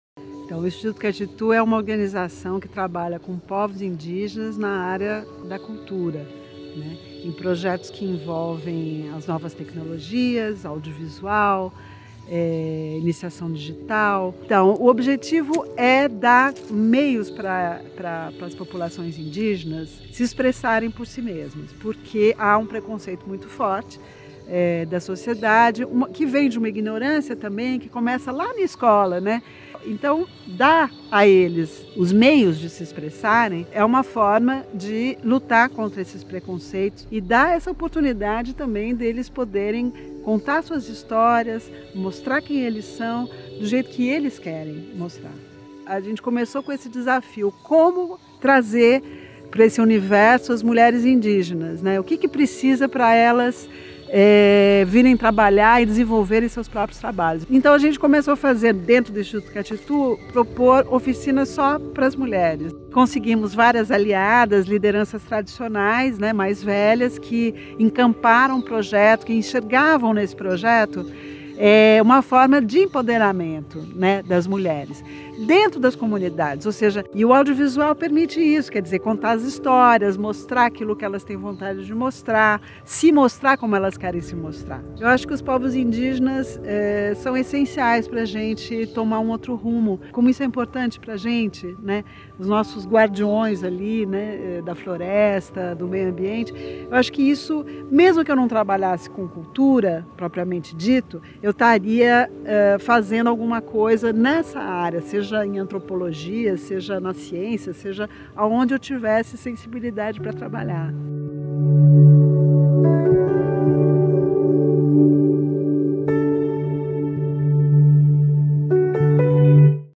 Prêmio CLAUDIA - Mari Côrrea - Categoria Cultura
Você conhece a história de sua família? É tão importante, não é? A Mari acredita tanto em preservar as raízes e os laços, que criou um incrível projeto para ensinar os índios a registrar sua cultura, história e hábitos.